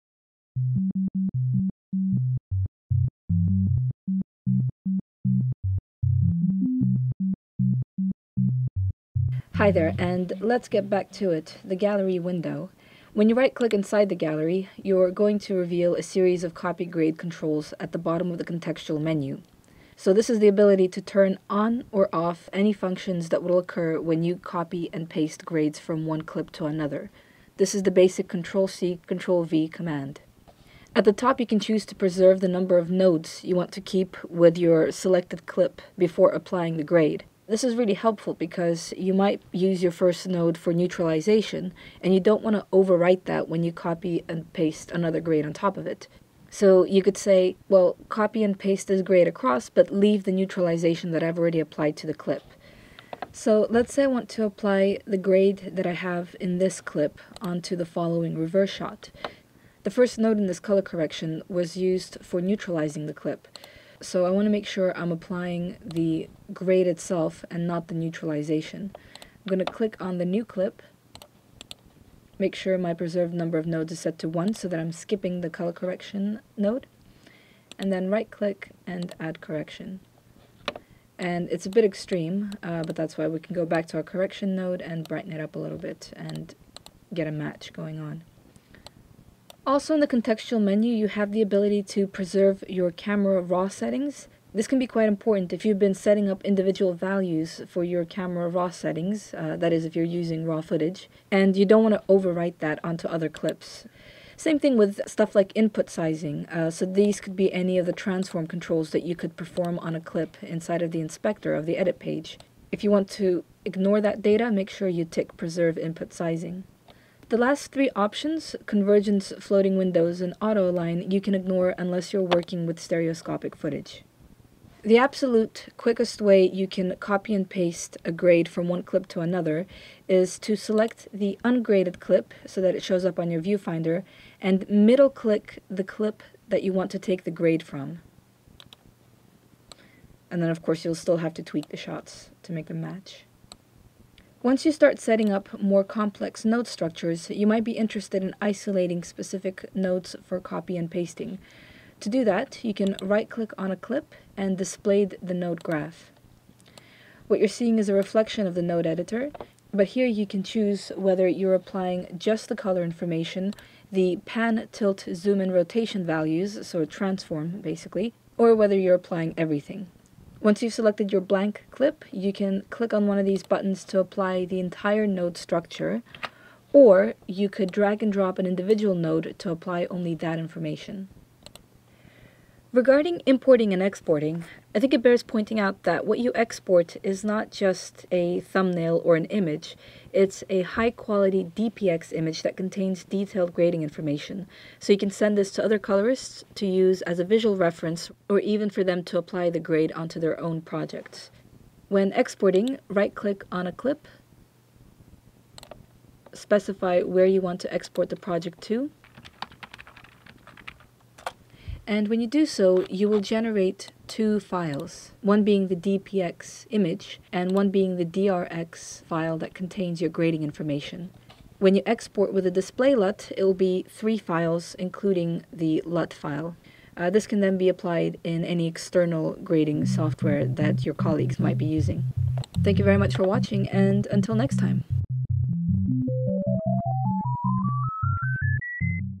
DaVinci Resolve 12 - 64 Copying Grades
Still in the Gallery palette, we look at the different ways we can copy grade and node information from one clip to another - from the simplest shortcuts, to the more sophisticated 'Copy Grade:' options in the contextual menu.

00:12 - 'copy grade:'
00:30 - preserve nodes
02:04 - middle click
02:24 - display node graph
03:01 - import/export stills (and LUTs)

Materials from Kerry (2015)
Director: Anthony Alleyne
DOP: Sam Parsons
Producer: Amber Lynch